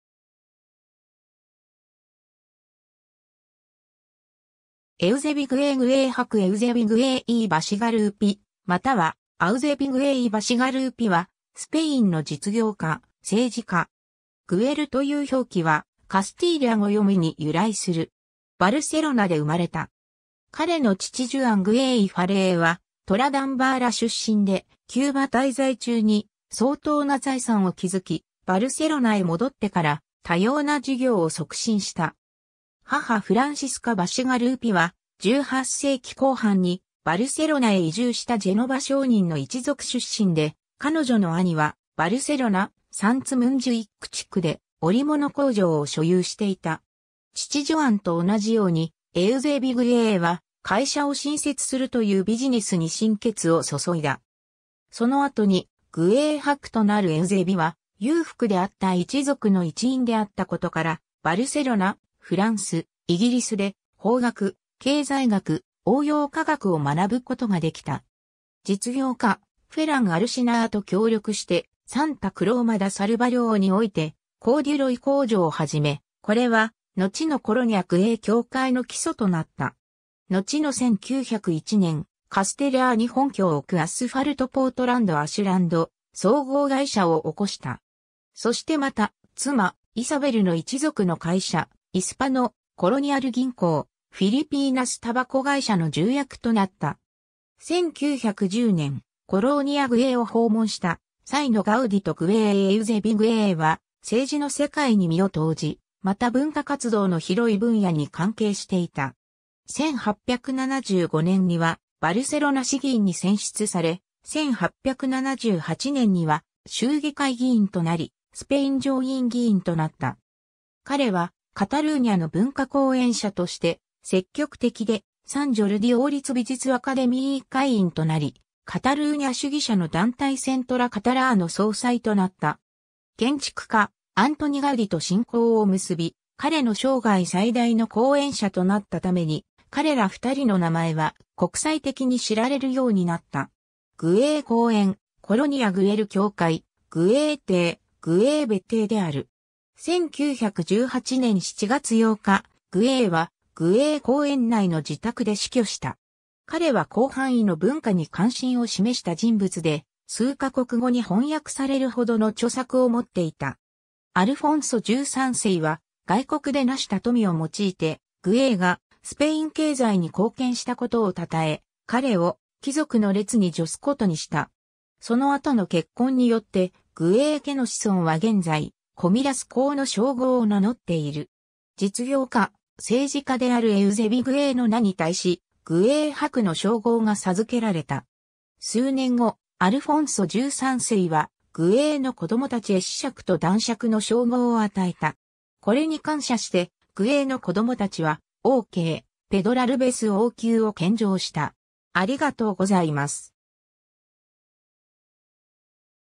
エウゼビ・グエイ
エウゼビ・グエイ グエイ伯エウゼビ・グエイ・イ・バシガルーピまたはアウゼビ・グエイ・イ・バシガルーピ（カタルーニャ語: Eusebi Güell i Bacigalupi, Comte de Güell, 1846年 — 1918年7月8日）は、スペインの実業家・政治家。
グエルという表記はカスティーリャ語読みに由来する。
 バルセロナで生まれた。
彼の父ジュアン・グエイ・イ・ファレーは、トラダンバーラ（タラゴナ県）出身で、キューバ滞在中に相当な財産を築き、バルセロナへ戻ってから多様な事業を促進した。
母フランシスカ・バシガルーピは、18世紀後半にバルセロナへ移住したジェノヴァ商人の一族出身で、彼女の兄はバルセロナ、サンツ・ムンジュイック地区で織物工場を所有していた。
 父ジョアンと同じようにエウゼビ・グエイは、会社を新設するというビジネスに心血を注いだ。
その後にグエイ伯となるエウゼビは、裕福であった一族の一員であったことから、バルセロナ、フランス、イギリスで法学、経済学、応用化学を学ぶことができた。
実業家フェラン・アルシナー(es)と協力して、サンタ・クローマ・ダ・サルバリョーにおいてコーデュロイ工場を始め、これはのちのコロニア・グエイ(es)教会の基礎となった（1891年）。
のちの1901年、カステリャー（en:Castellar de n'Hug）に本拠をおくアスファルト・ポートランド・アシュランド総合会社を興した。
そしてまた、妻イサベル（イスパノ・コロニアル銀行を創設したロペス財閥、コミーリャス侯爵家出身）の一族の会社、イスパノ・コロニアル銀行、フィリピーナス・タバコ会社（Compañía General de Tabacos de Filipinas）の重役となった。
 1910年、コローニア・グエイを訪問した際のガウディとグエイ エウゼビ・グエイは政治の世界に身を投じ、また文化活動の広い分野に関係していた。
1875年にはバルセロナ市議員に選出され、1878年には州議会議員となり、スペイン上院議員となった。
彼はカタルーニャの文化後援者として積極的で、サン・ジョルディ王立美術アカデミー会員となり、カタルーニャ主義者の団体セントラ・カタラーの総裁となった。
 建築家アントニ・ガウディと親交を結び、彼の生涯最大の後援者となったために、彼ら2人の名前は国際的に知られるようになった。
グエイ公園、コロニア・グエル教会、グエイ邸、グエイ別邸である。
1918年7月8日、グエイはグエイ公園内の自宅で死去した。
彼は広範囲の文化に関心を示した人物で、数カ国語に翻訳されるほどの著作を持っていた。
 アルフォンソ13世は、外国で成した富を用いてグエイがスペイン経済に貢献したことを讃え、彼を貴族の列に序すことにした。
その後の結婚によって、グエイ家の子孫は現在コミリャス侯の称号を名乗っている。
実業家、政治家であるエウゼビ・グエイの名に対し、グエイ伯の称号が授けられた。
数年後、アルフォンソ13世はグエイの子供たちへ子爵と男爵の称号を与えた。
これに感謝して、グエイの子供たちは、王家へペドラルベス王宮（es:Palacio Real de Pedralbes、グエイ邸パヴィリオン内）を献上した。